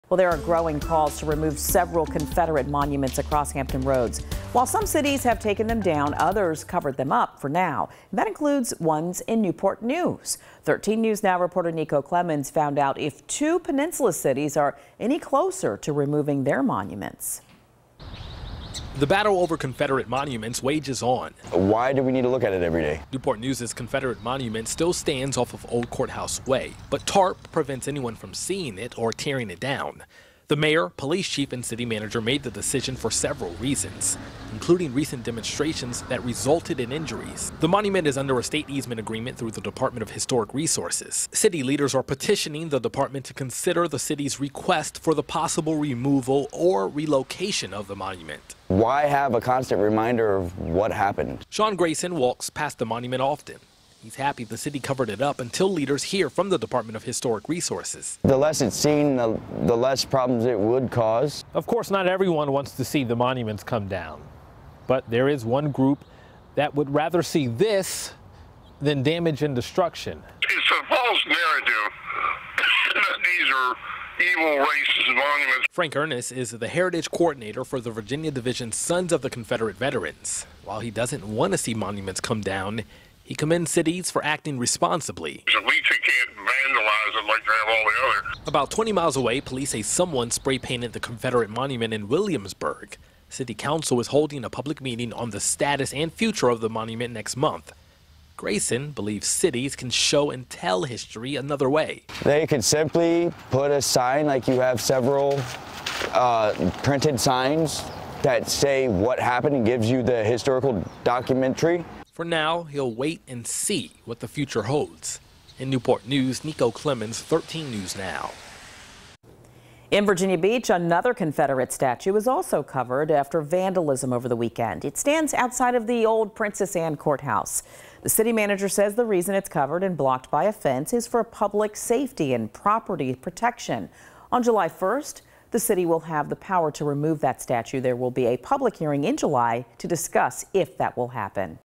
Calls to remove Confederate monuments in Newport News
While some cities have taken the Confederate monuments down, others covered them up  for now. That includes ones in Newport News.